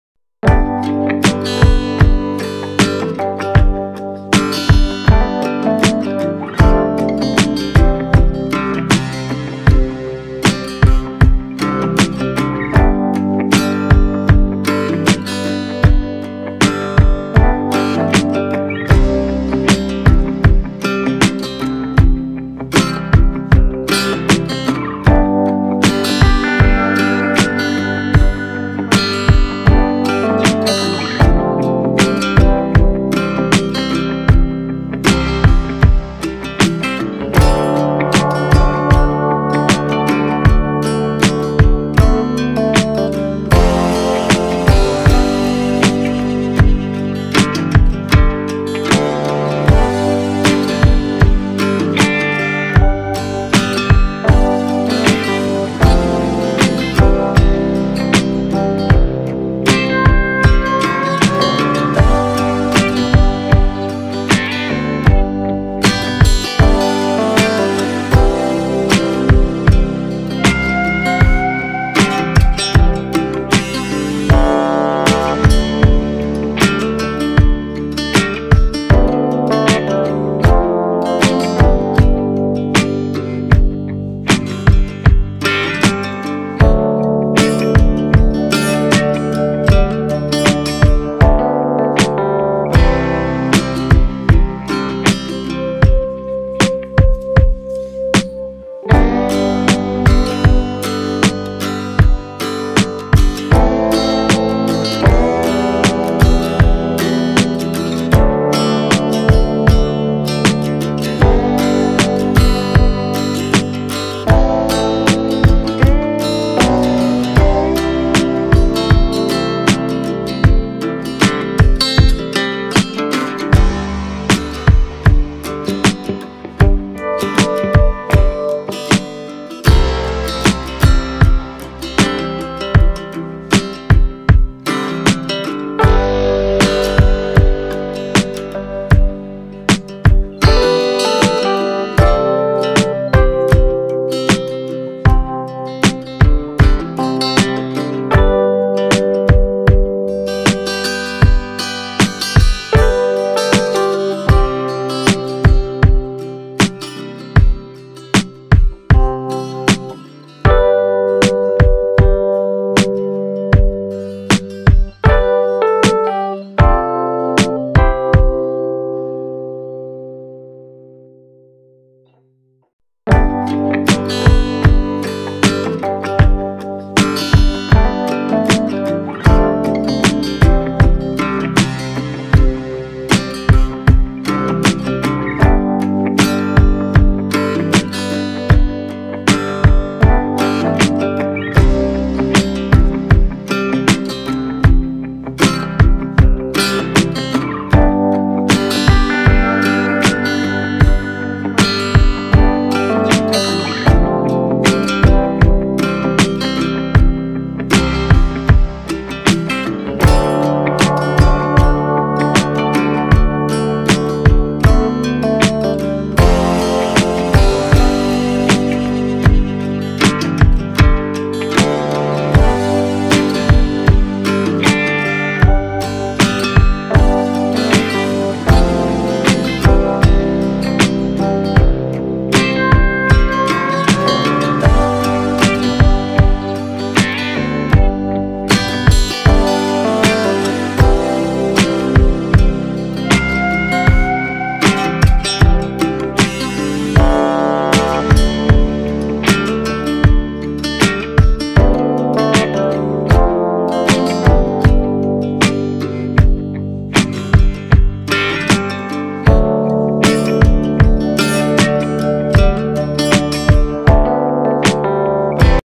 Coster Making idea | Coster panting | How-to make costar
Hello everyone I hope you all are doing well.
Hindi video I will show you how to make poster from west cardboard I have printed them with Bird art. you can try the sum or try out any other painting. or stick any pictures with glue and sale with burnish I hope you enjoy the video and if you do please hit the like button if you are watching my video for the first time please subscribe my channel.